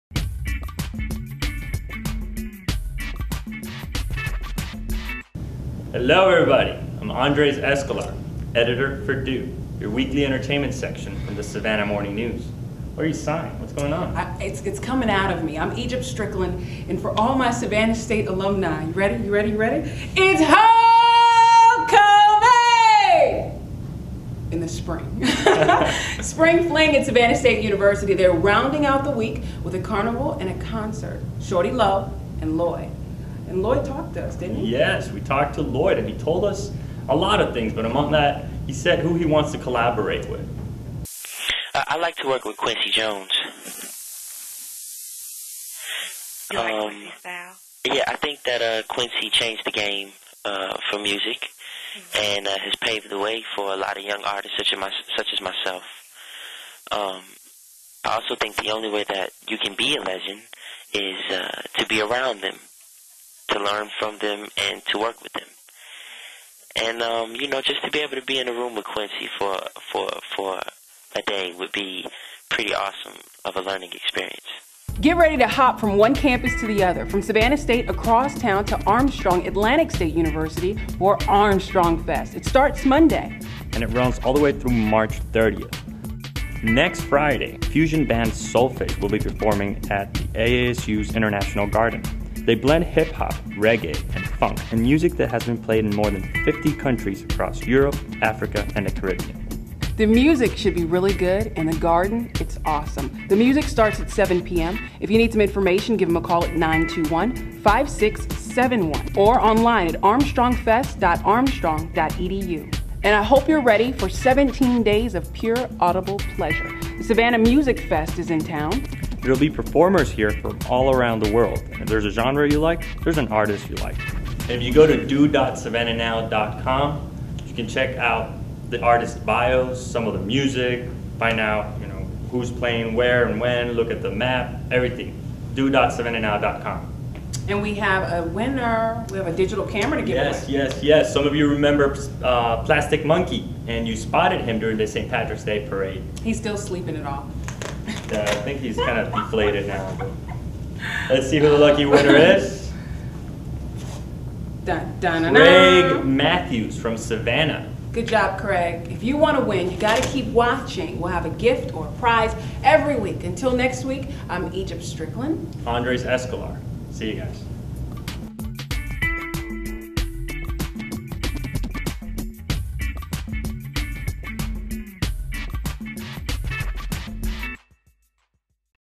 Andres & Egypt (3/20/08)
Andres & Egypt discuss the upcoming concernt at Savannah State featuring Lloyd, plus AASU gets ready for spring fun. You can also get details on the Savannah Music Festival.